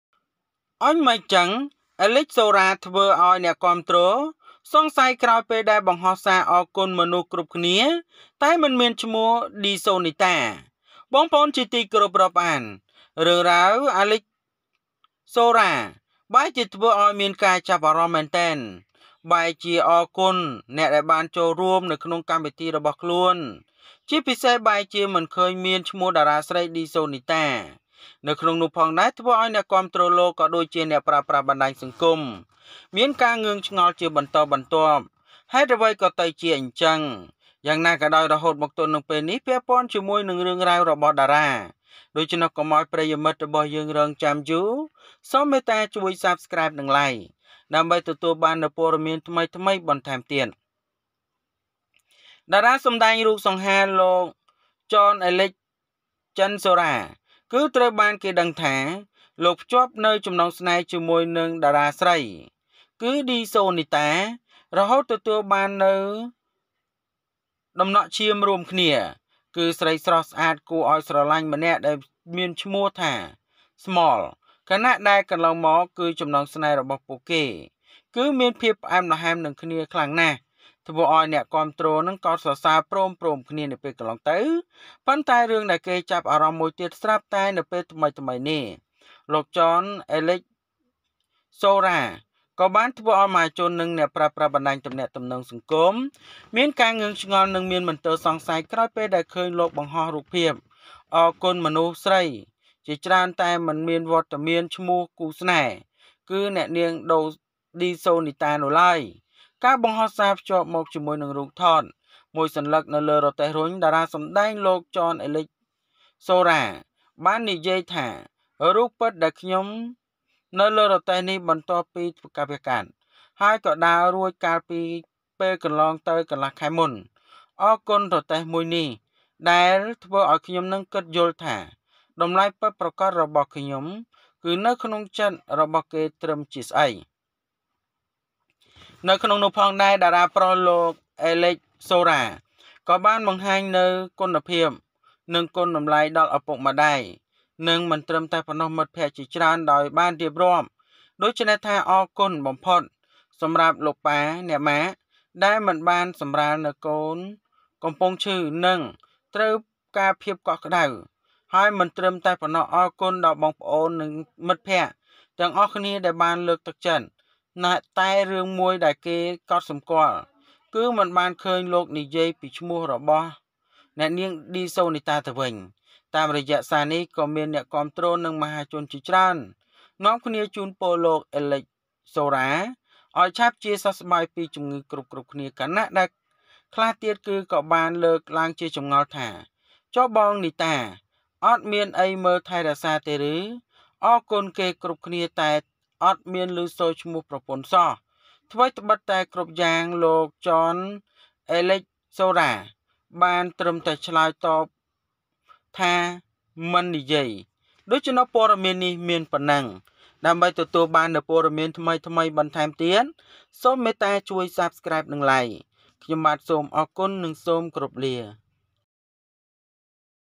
អេ​ឡិច ហ្សូ​រ៉ា ធ្វើ​ឲ្យ​អ្នក​គាំទ្រ​សង្ស័យ​ ក្រោយ​បង្ហោះ​សារ​អរគុណ​មនុស្ស​តែមិនមាន ឌី សូ​នី​តា
អុញម៉េចចឹង! អេ​ឡិច ហ្សូ​រ៉ា ធ្វើ​ឲ្យ​អ្នក​គាំទ្រ​សង្ស័យ​ ក្រោយ​បង្ហោះ​សារ​អរគុណ​មនុស្ស​គ្រប់គ្នា​ តែ​មិន​មាន​ឈ្មោះ ឌី សូ​នី​តា Oh, how!  Alex Zora Makes Fans Suspicious After Posting Thank You Message to Everyone But Not Dysonita.